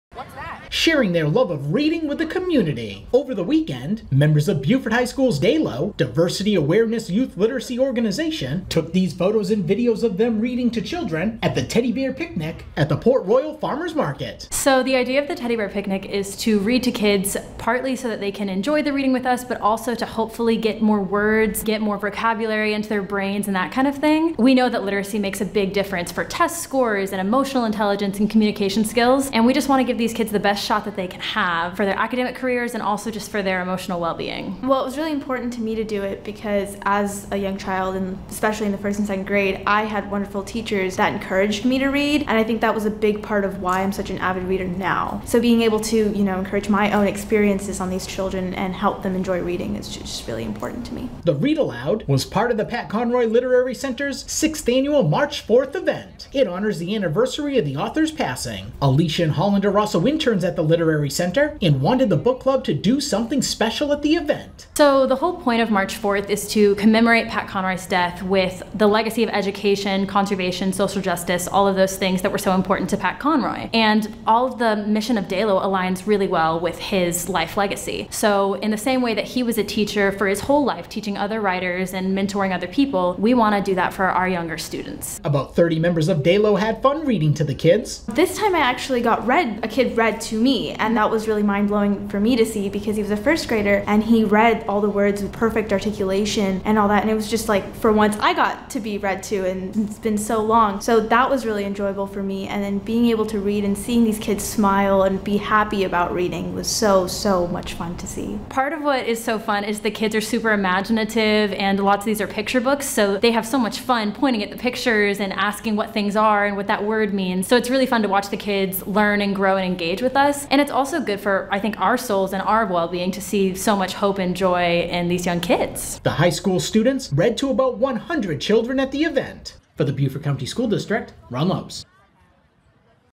Creating a Community of Readers
Beaufort High School's DAYLO (Diversity Awareness Youth Literacy Organization) read to children at a Teddy Bear Picnic during the Pat Conroy Literary Center's 6th Annual March Forth event.